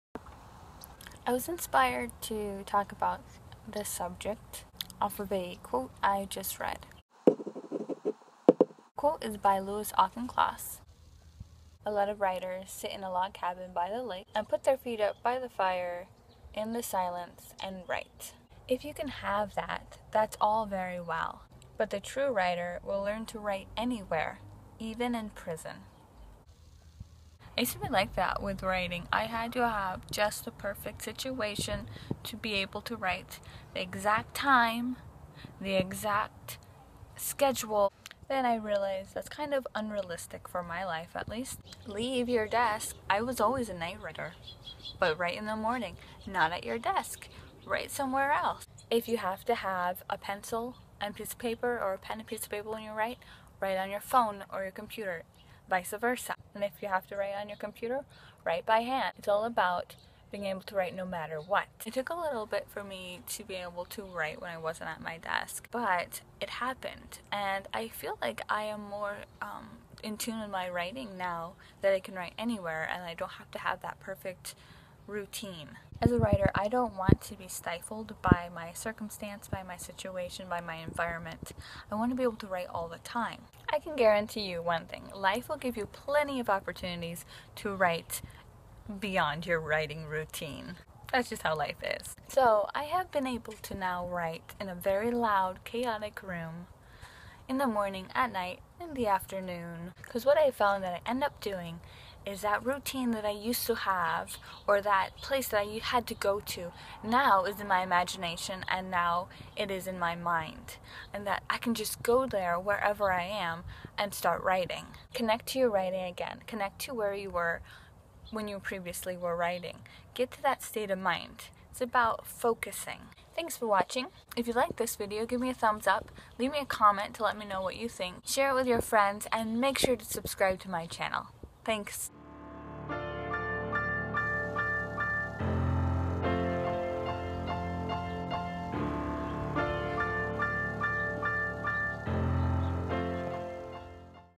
Write anywhere, anytime
Vlog 10 - I was inspired by a quote by Louis Auchincloss to talk about learning to be able to write anywhere, anytime.

"A lot of writers ... sit in a log cabin by the lake and put their feet up by the fire in the silence and write. If you can have that that's all very well, but the true writer will learn to write anywhere -- even in prison."
- Louis Auchincloss

Quotation hands: Juleah Kaliski